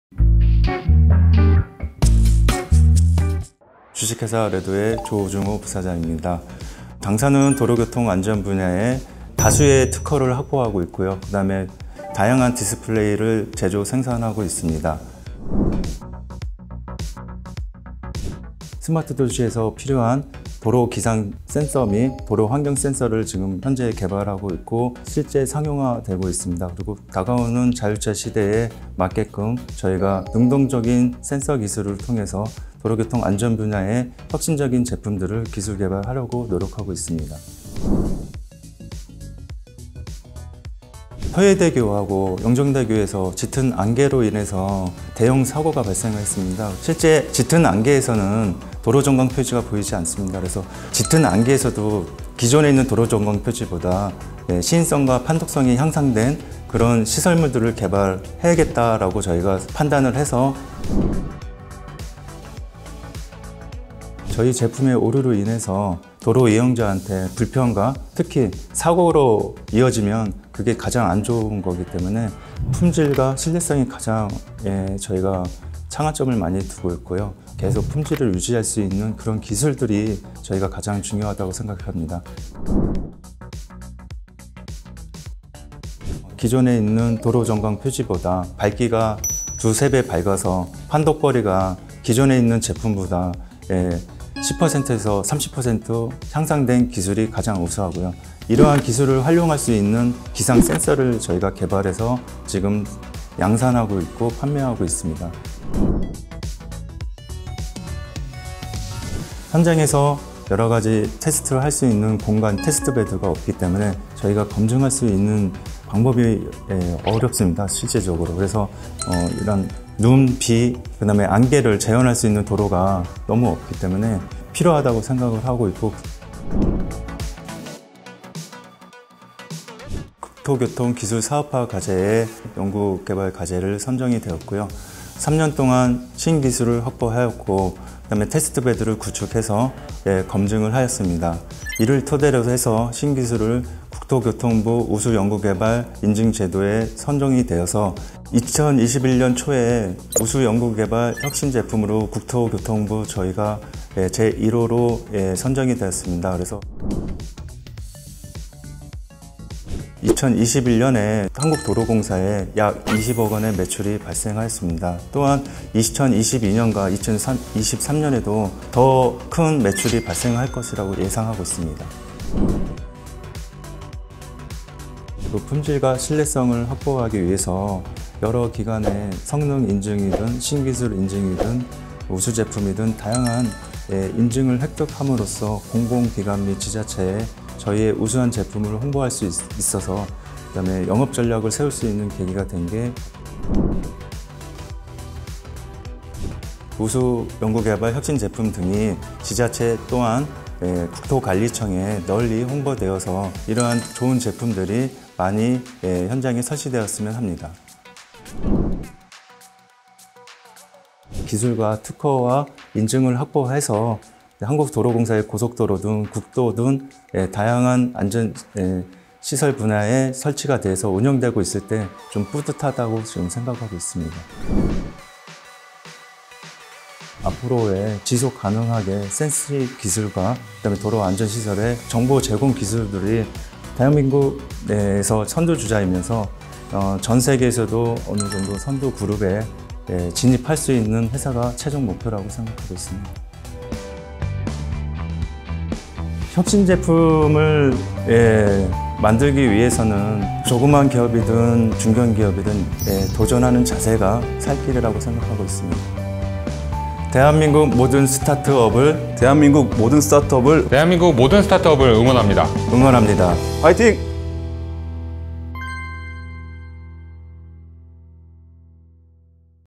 국토교통기업지원허브 인터뷰#4(래도)
요즘 기업 픽 인터뷰 (래도편)
도로교통안전 베테랑 첨단 IT 기업! '래도' 인터뷰